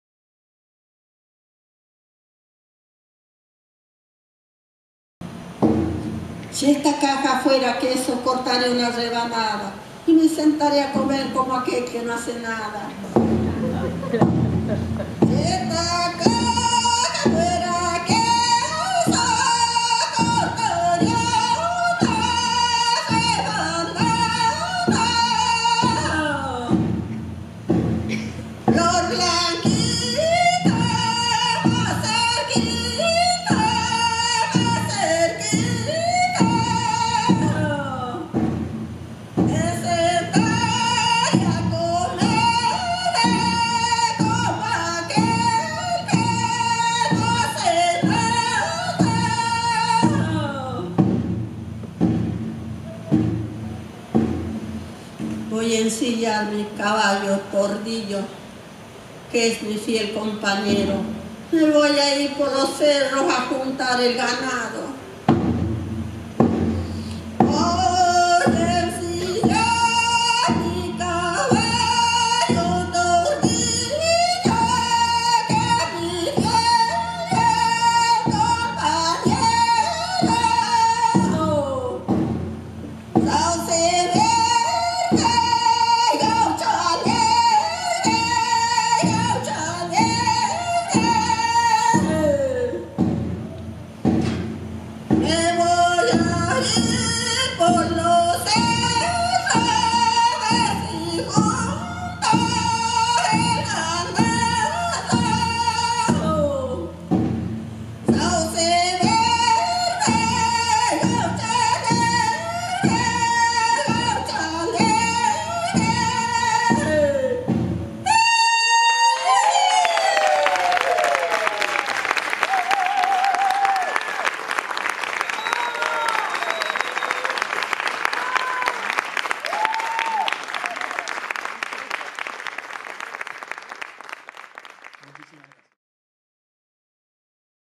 Homenaje a Leda Valladares en el Centro Cultural Haroldo Conti - Participación de Dorotea Tolaba.
Durante el homenaje por los 100 años de Leda Valladares realizado el 19 de diciembre de 2019 en el Centro Cultural Haroldo Conti de la ciudad de Buenos Aires, participa Dorotea Tolaba.

La Savia Inquieta Producciones
Cámaras: Gustavo Bustamante, Alejandro Los Santos, Guillermo Meana Morales y Daniel Rojas.